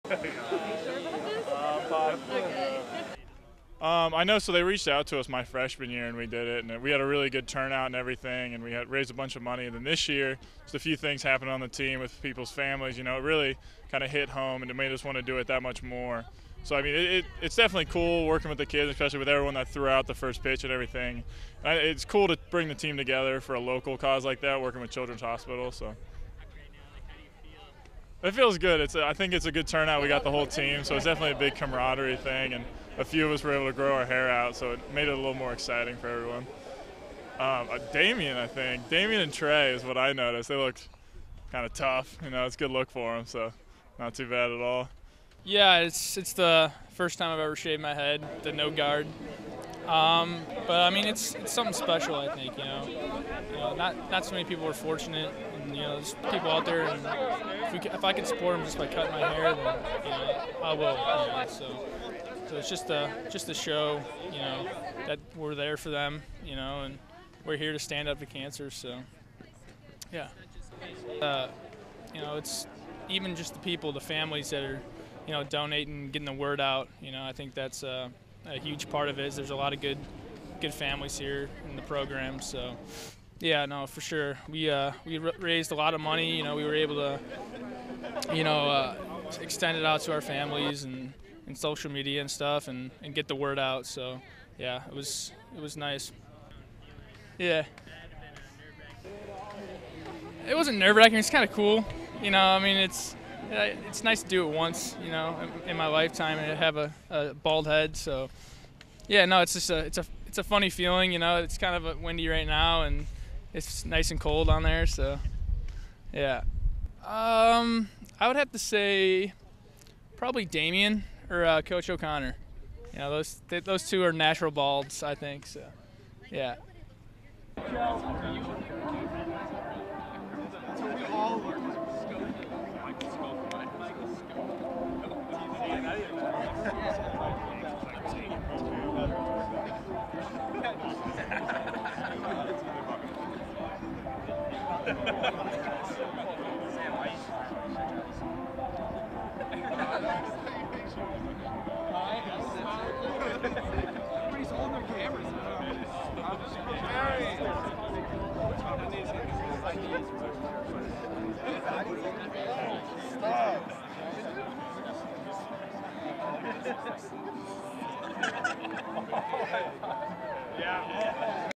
Baseball Shaves Hair for Vs. Cancer Foundation
The Musketeers raised over $13,000 in support of the organization and the team let some locks fly after Sunday's game against Morehead State.